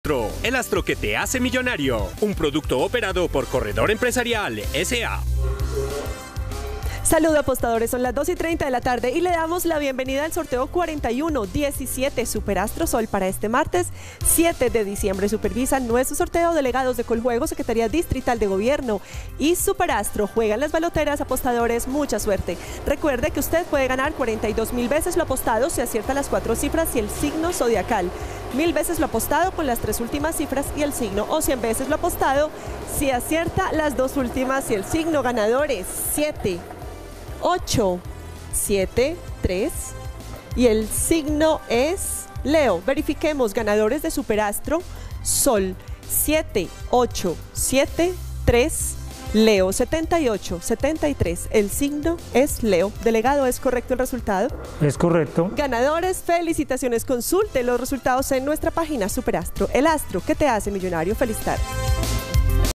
ASTRO SOL Resultado ASTRO SOL del martes 07 de diciembre de 2021 ✅🥇🔥💰
Hola buenas tardes, te presentamos los resultados de las loterías y chances en Colombia, vamos con el Resultados del Chance SUPER ASTRO SOL del martes 07 de diciembre de 2021.

Resultados de astro

Resultado astro SOL 07 de diciembre
Resultado LUNA 07 de diciembre
Resultado ASTRO SOL MARTES 07 de diciembre